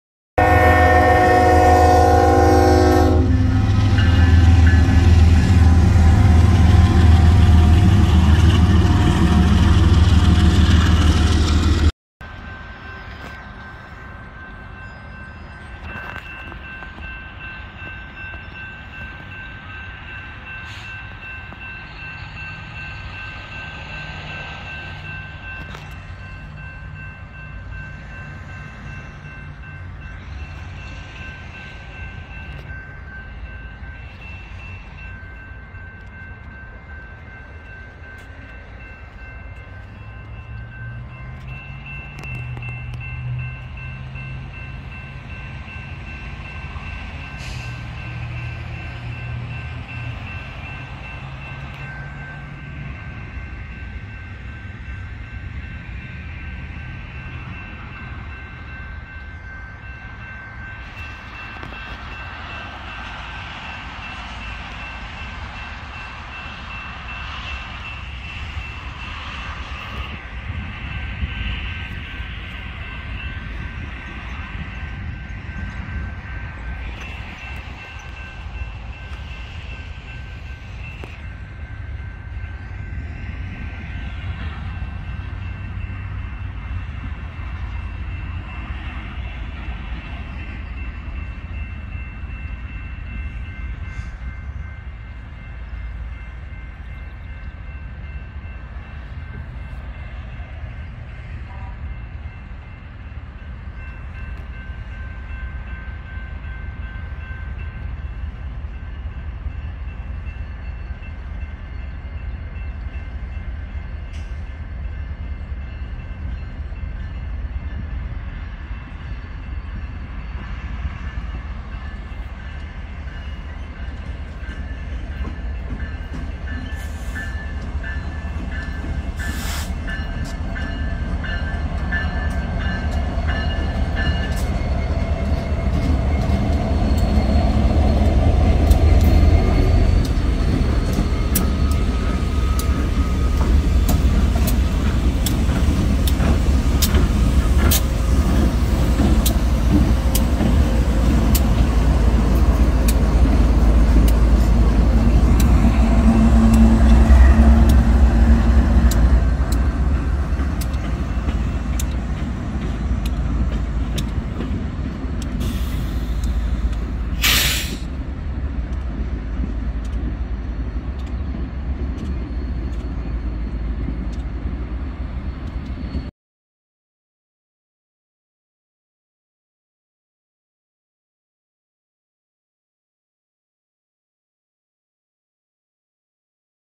4 Engines (2 BNSF & 2 OMLX) rolling south through Montgomery, Illinois
I caught these 4 engines switching off the main and heading south down a spur line through Montgomery, Illinois.